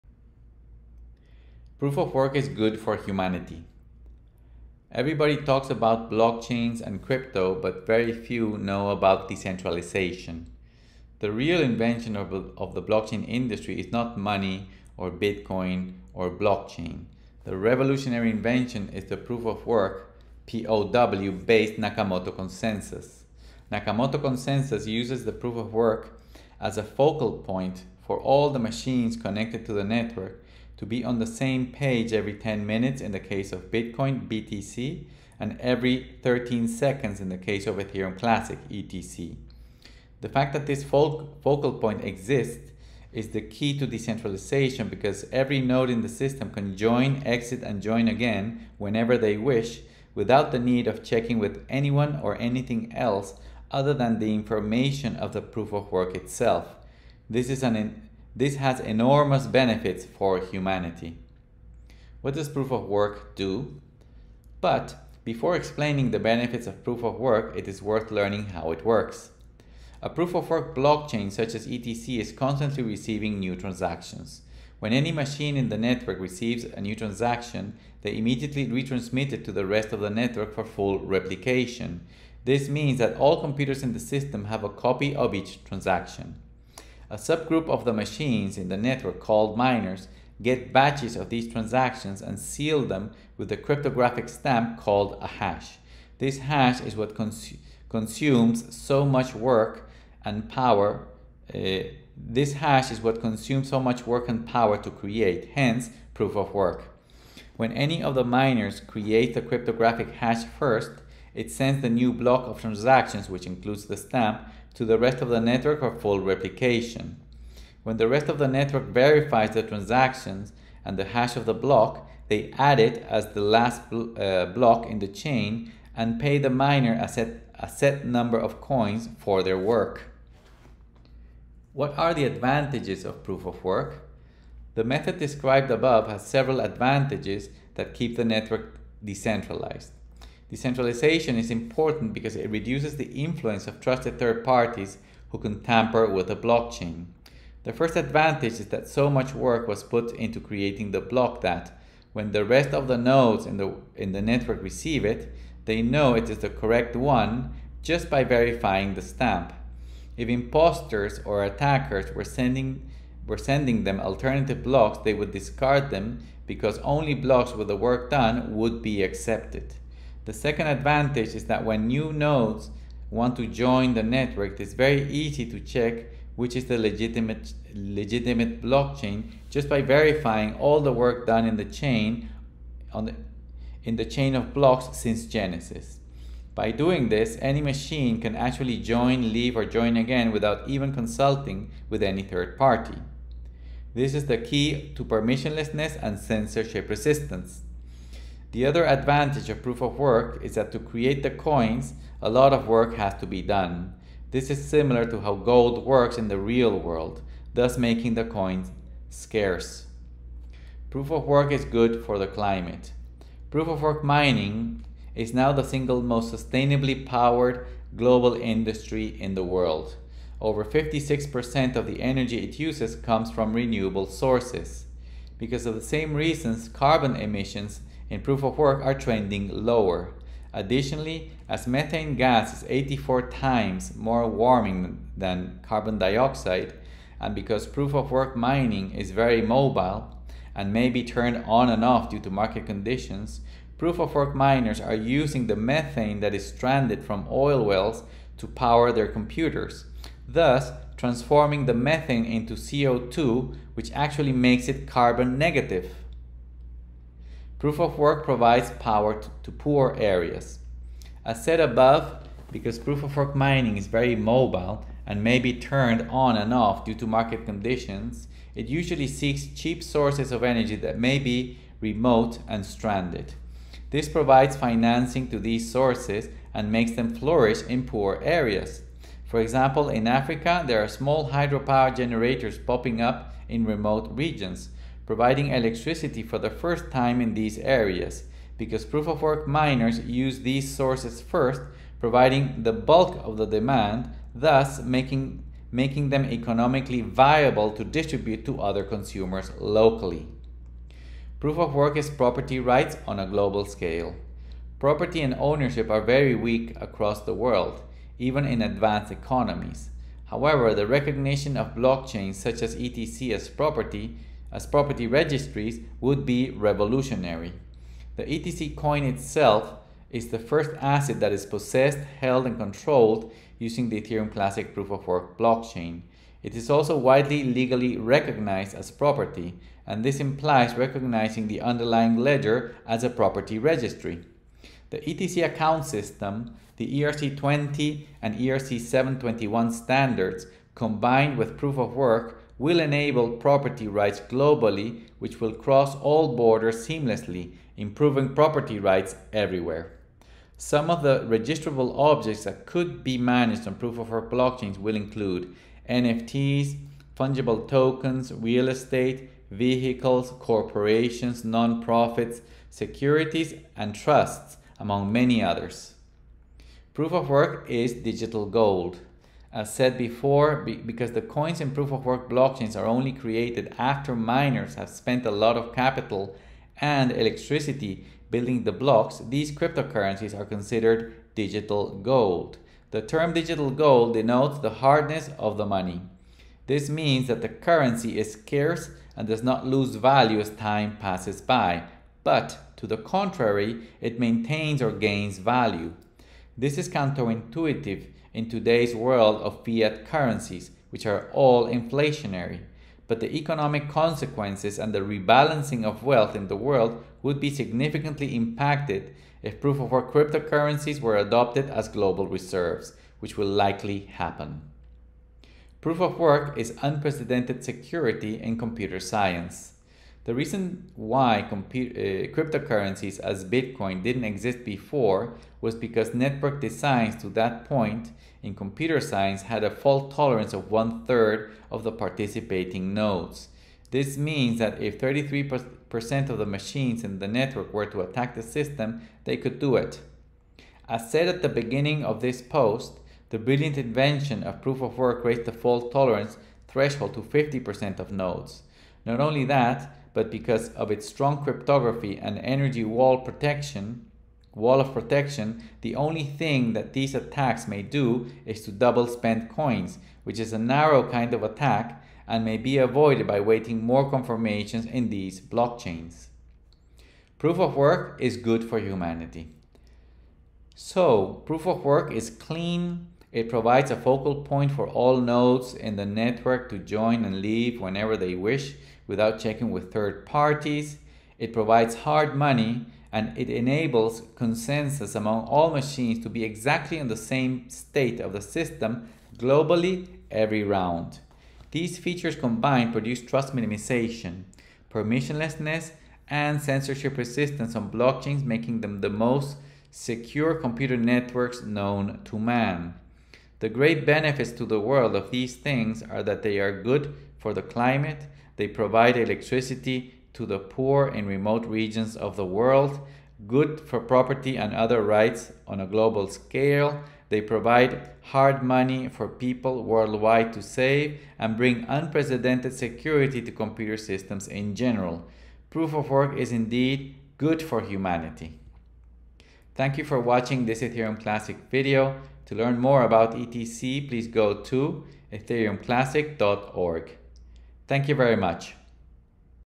Proof of Work Is Good for Humanity
Everybody talks about blockchains and crypto, but very few know about decentralization.

The real invention of the blockchain industry is not “money” or “Bitcoin” or “blockchain”.

The revolutionary invention is the proof of work (POW) based Nakamoto Consensus.

Nakamoto Consensus uses the POW as a focal point for all the machines connected to the network to be on the same page every 10 minutes, in the case of Bitcoin (BTC), and every 13 seconds, in the case of Ethereum Classic (ETC).

The fact that this focal point exists is the key to decentralization because every node in the system can join, exit, and join again whenever they wish, without the need of checking with anyone or anything else other than the information of the proof of work itself.

This has enormous benefits for humanity.